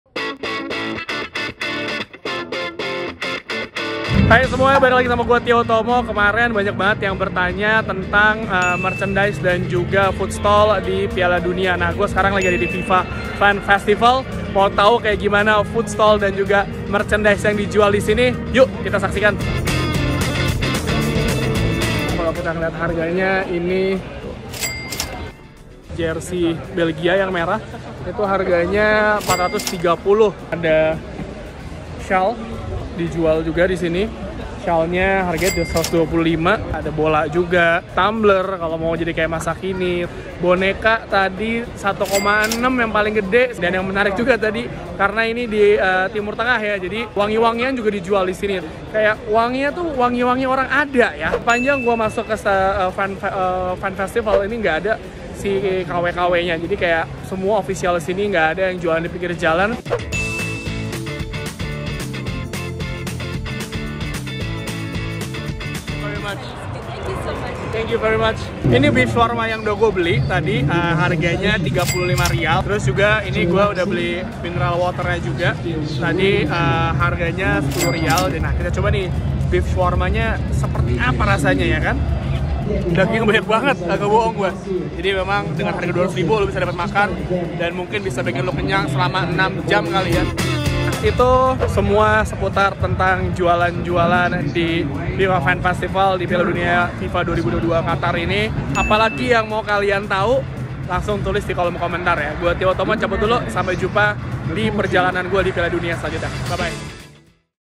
Belanja Merchandise dan Jajan Camilan Piala Dunia Qatar, Ada Apa Aja?
Merchandise dan jajanan apa aja yang dijual di Piala Dunia Qatar? Dan ada nggak sih yang jualan merchandise KW atau imitasi di pinggir jalan?

Tio Utomo bawa kalian keliling FIFA Fan Festival untuk lihat apa saja yang bisa dibeli dan berapa kisaran harganya!

Menurut kalian jajanan dan jualan harga segini, worth it dibeli nggak?